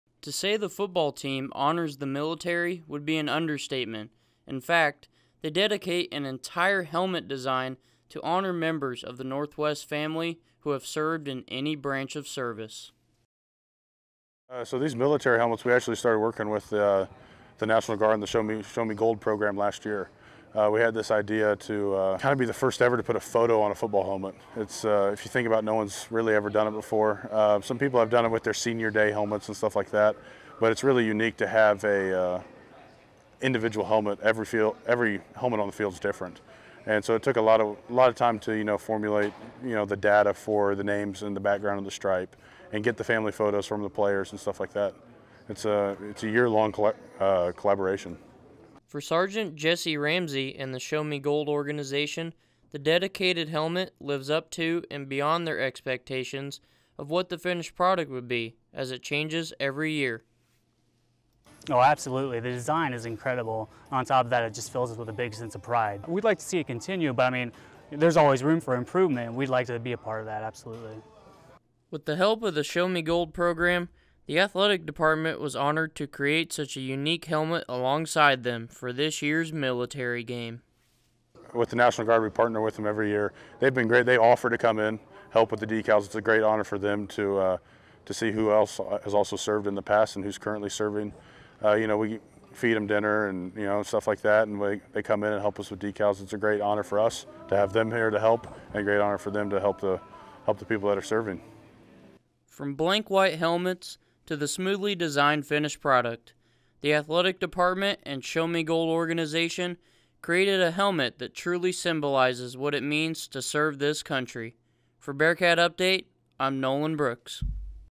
Northwest Missouri State Military Helmet Feature
This is a self shot and edited video I created to showcase the very unique and slickly designed military appreciation helmets for the Northwest Missouri State Football team.